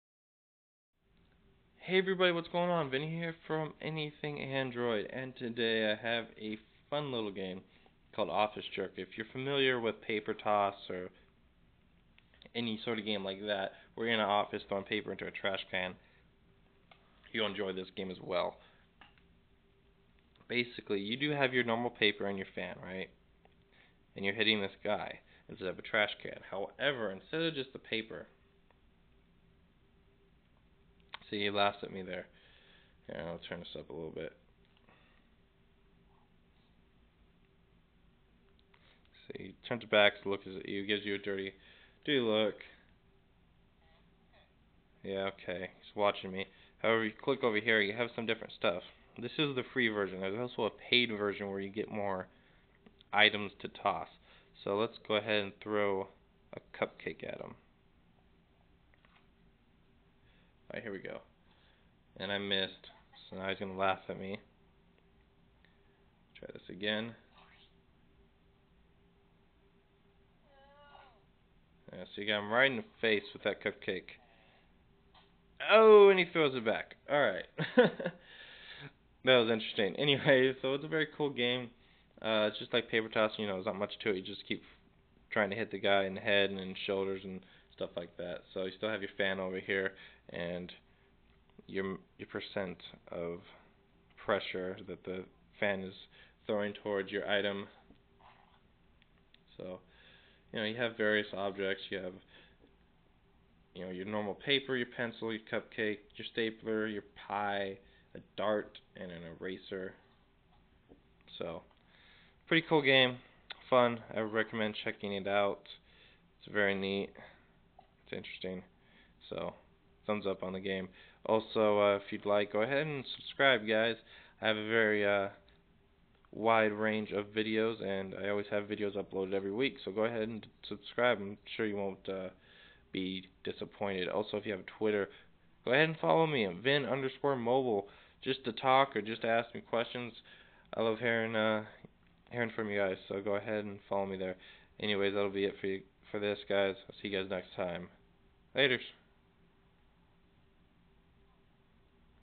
FUNNY Android GAME Review Office Jerk
Fun game, stays interesting, and helps kill some time!
Recommended to download.